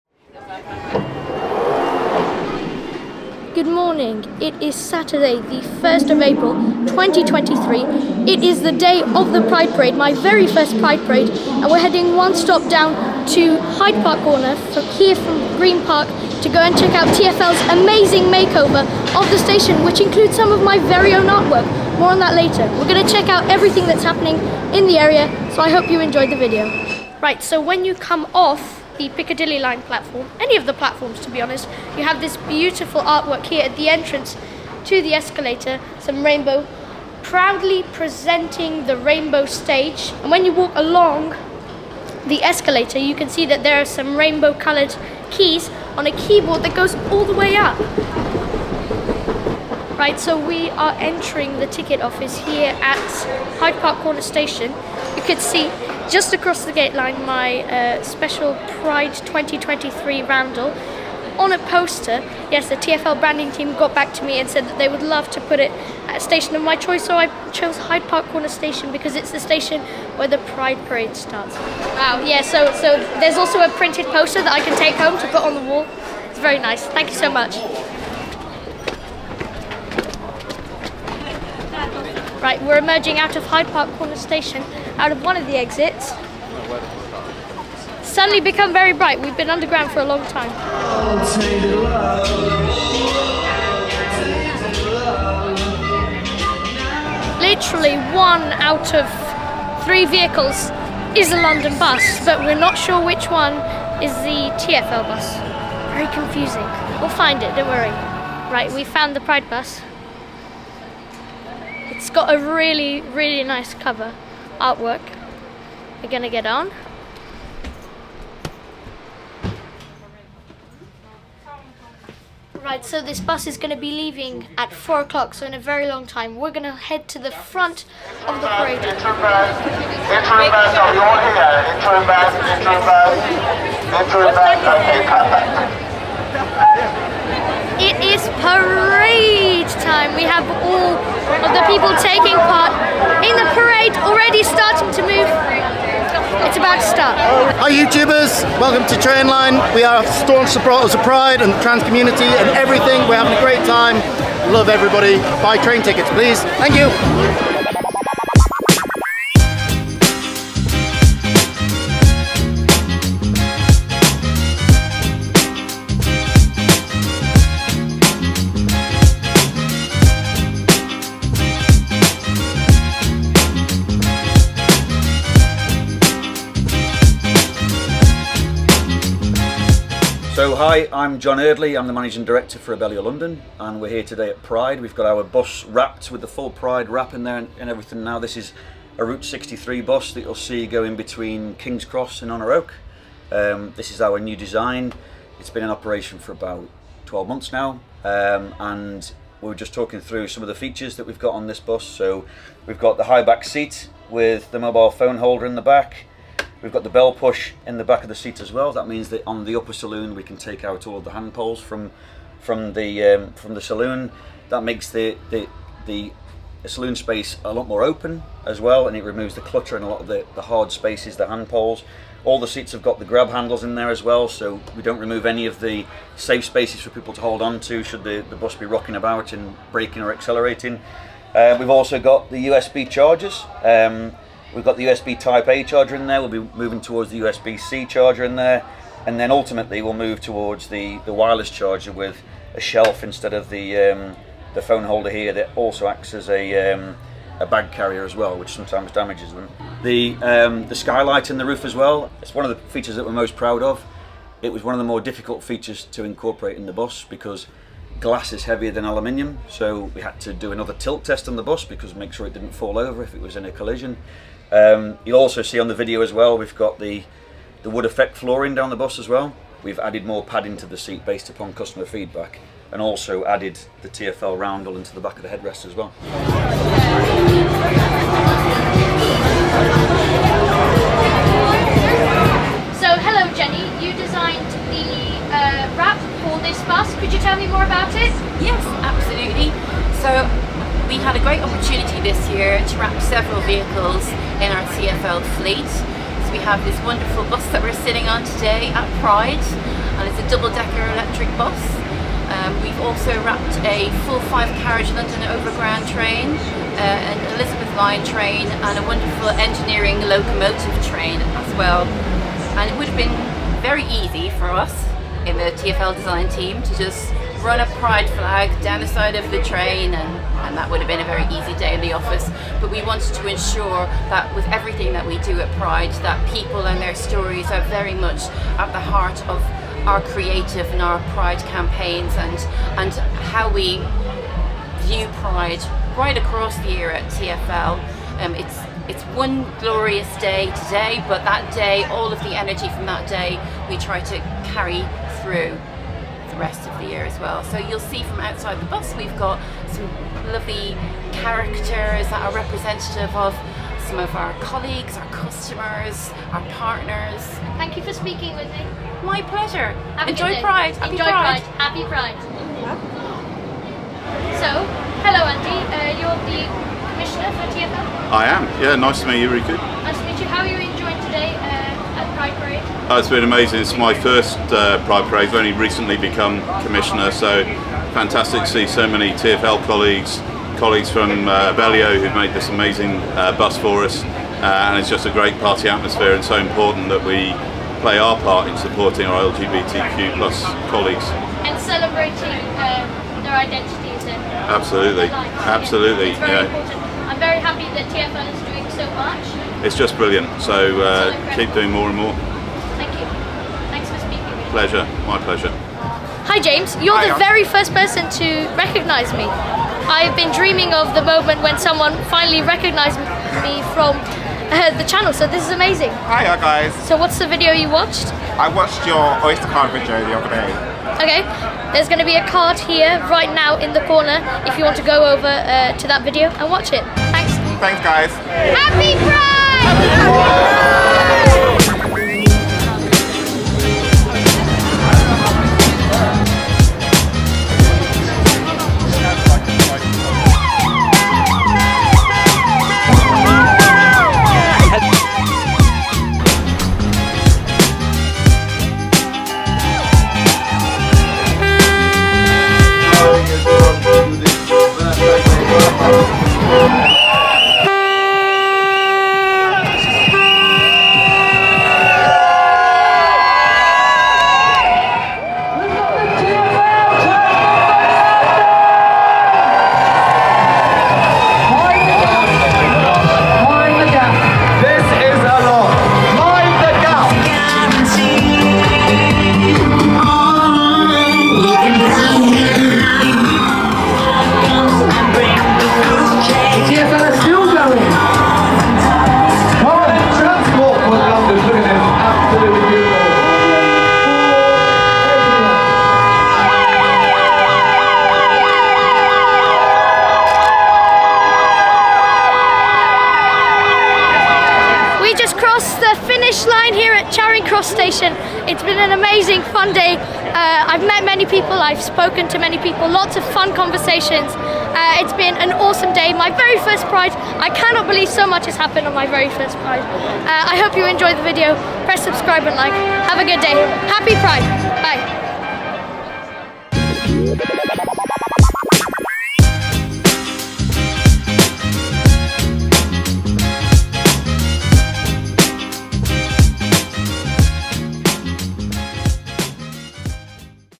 TFL at Pride
The very first time at the pride parade, a lot happened. I saw my pride2023 roundel poster displayed at Hyde Park Corner Station, met many incredible people from Transport for London who all play a vital role in keeping London moving, and joined them, marching from Marble Arch to Charring Cross, as we celebrated the LGBTQIA+ community. This was an incredible experience for me as a genderlfuid individual which brought me much joy and a sense of community. This video is a summary of this extraordinary day. I hope you enjoy it.

Thank you to @TransportforLondon  for giving me the opportunity to march with them, and to Andy Lord, Jenney De Sousa and Jon Eardley for being such amazing interviewees.

Go to prideinlondon.org for more information on @prideinlondon
This video is shot in 4K.